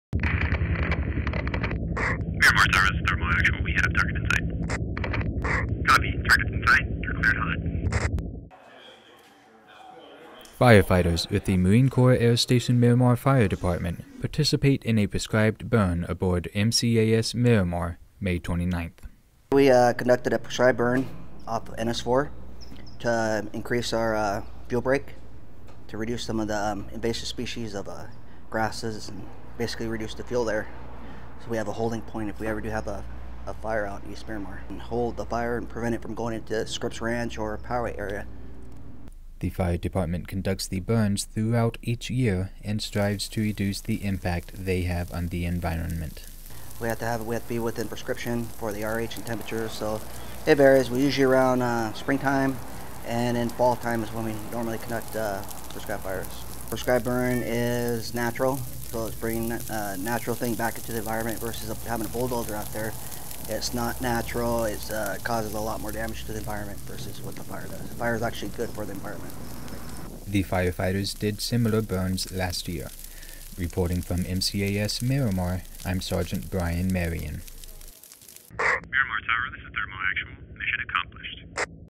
Prescribed Burn
Firefighters with the Marine Corps Air Station Miramar Fire Department perform a prescribed burn aboard MCAS Miramar May 29.